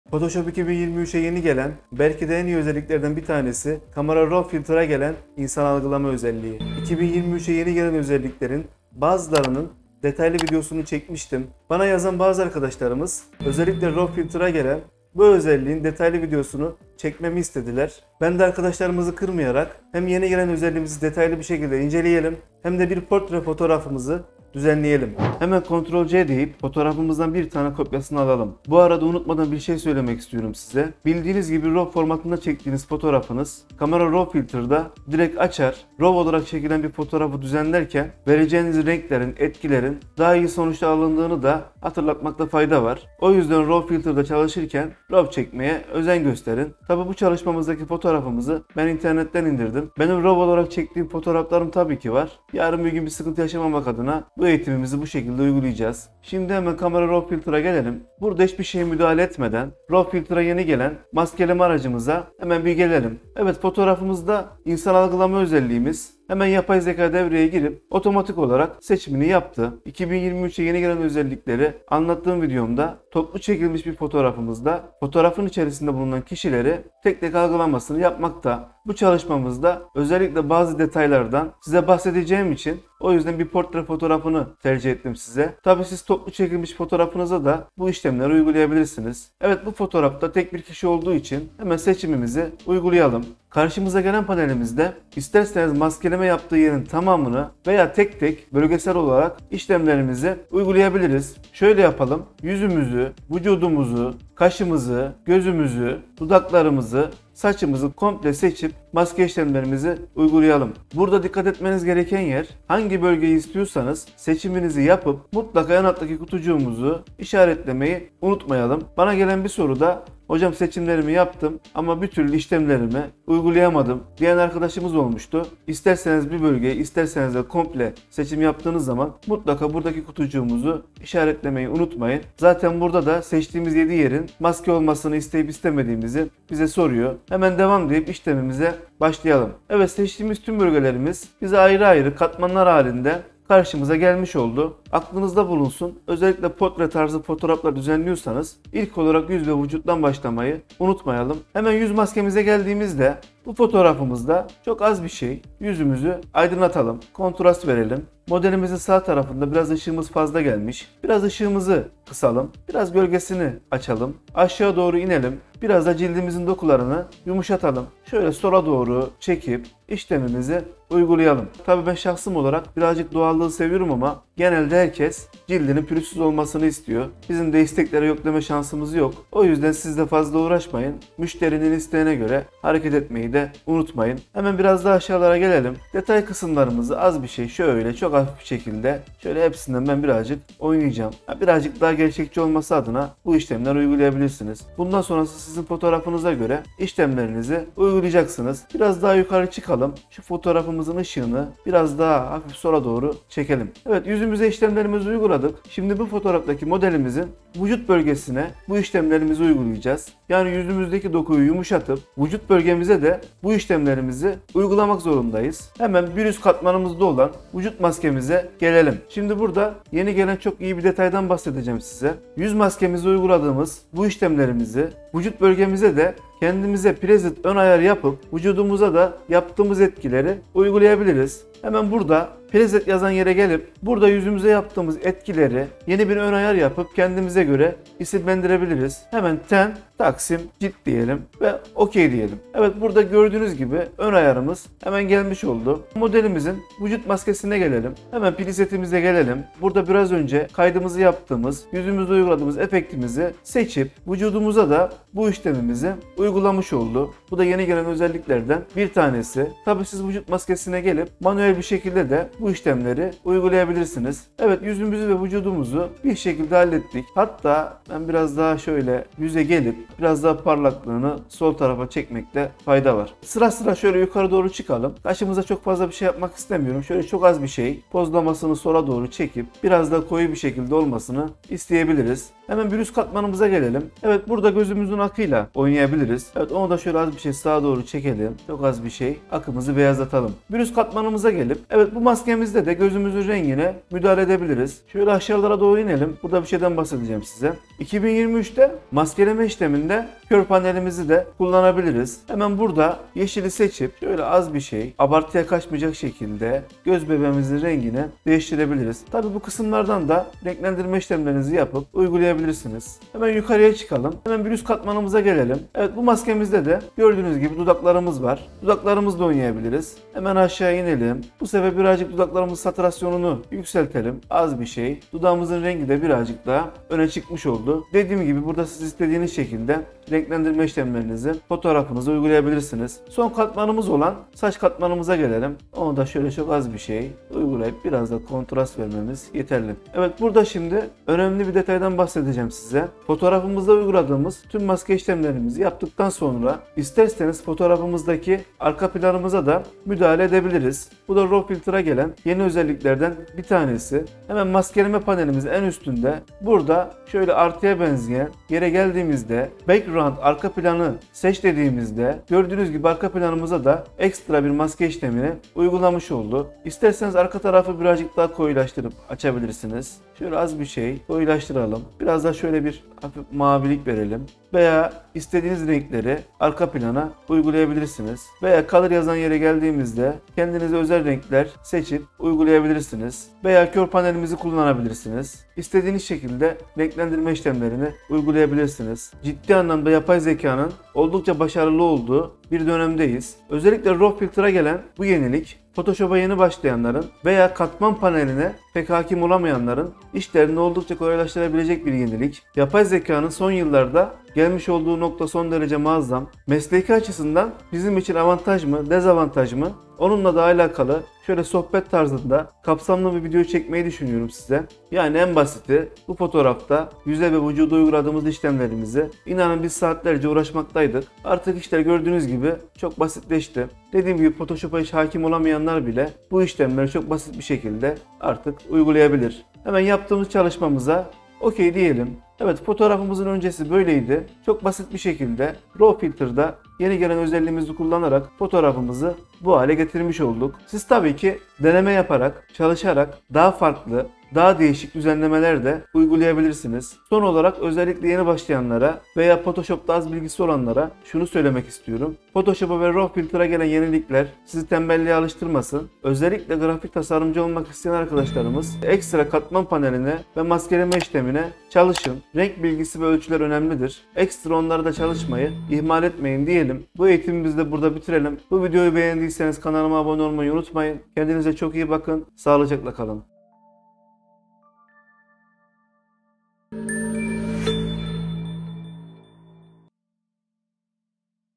ADOBE DELİRDİ! Photoshop Camera Raw Filter 2023
Herkese selamlar; Photoshop Camera Raw Filter 2023'e yeni gelen insan algılama özelliğinin detaylı anlatımını izleyebilirsiniz.

İyi seyirler.